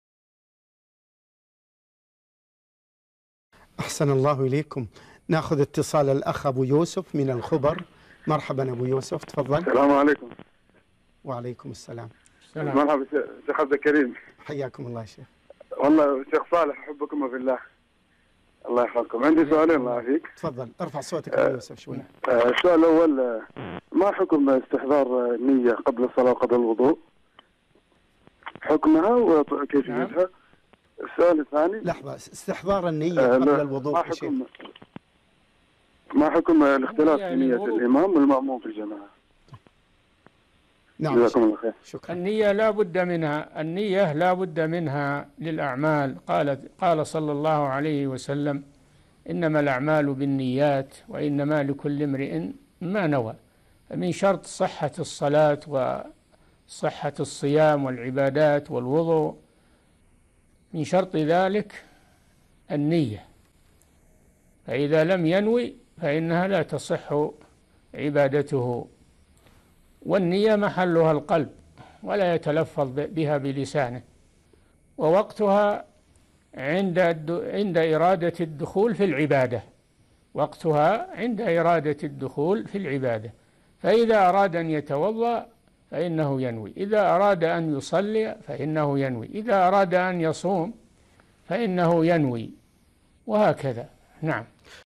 استحضار النية قبل كل عمل - الشيخ صالح الفوزان
نص السؤال: ما حكم استحضار النية قبل الصلاة قبل الوضوء، حكمها؟ رابط الحلقة كاملة
السيرة الذاتية :
للاشتراك في قناة الشيخ صالح الفوزان
لمشاهدة حلقات فتاوى على الهواء كاملة

رئيس الفريق
لمشروع كبار العلماء